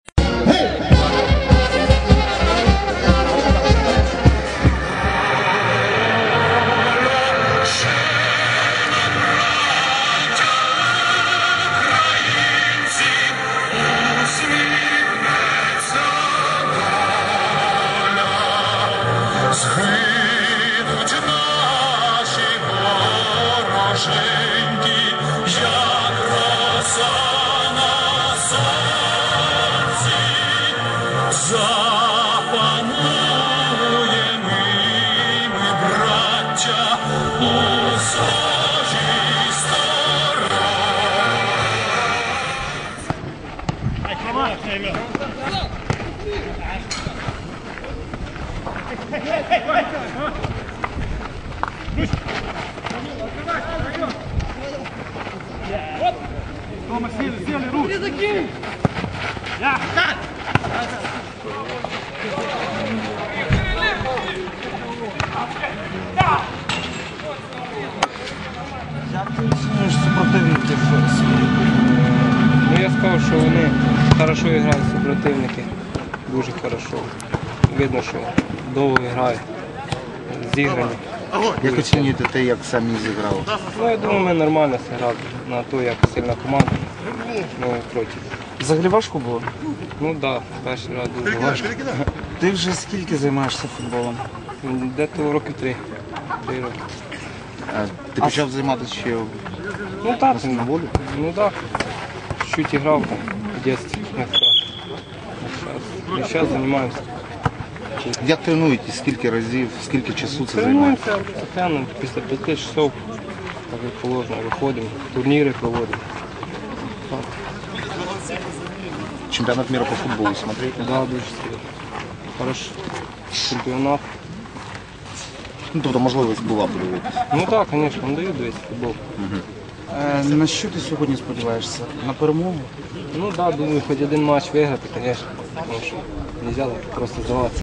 зеки играют в футбол.wmv.AVI
Единственный в Украине чемпионат по футболу между зэками и бизнесменами проводится в Могилев-Подольской исправительной колонии.